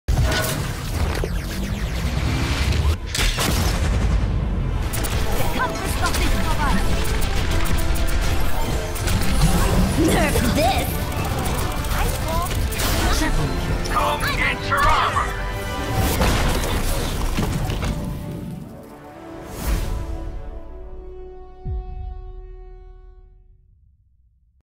This Is Why Mei Is Evil
Mei left them all for dead and did not even care, or blink. EVIL.